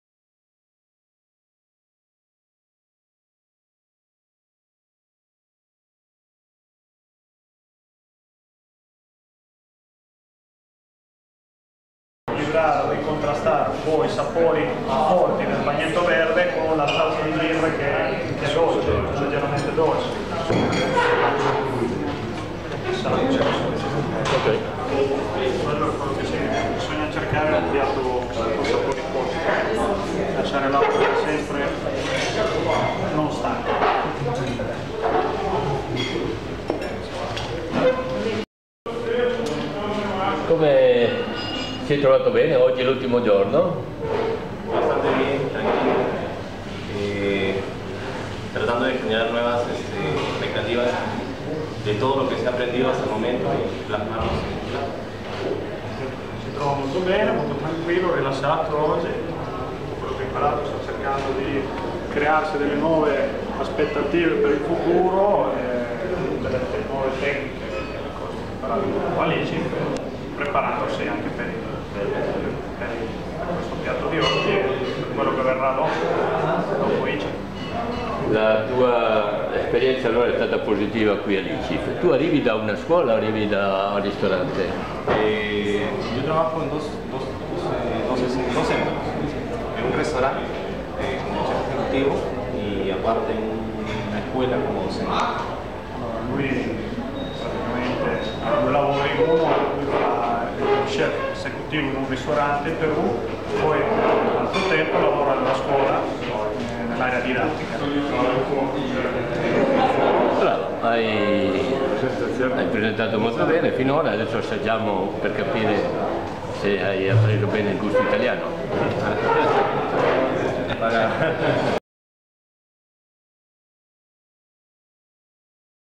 Stakeff Fernandez Boris Yeugene - ICIF - Escuela de Cocina Italiana
Stakeeff Fernandez Boris Yeugene, estudiante de Perú, se ha dado cuenta de la Acciughe farcite con bagnetto verde e fritto di baccalà. Recetas tradicionales y modernos-creativas para una educación completa extensión educativa!
El ICIF es una de las mejores escuelas de cocina italiana en el mundo que capacita a los estudiantes en el conocimiento de la comida italiana y el vino: una experiencia fundamental para aquellos que quieren mejorar profesionalmente ya nivel internacional.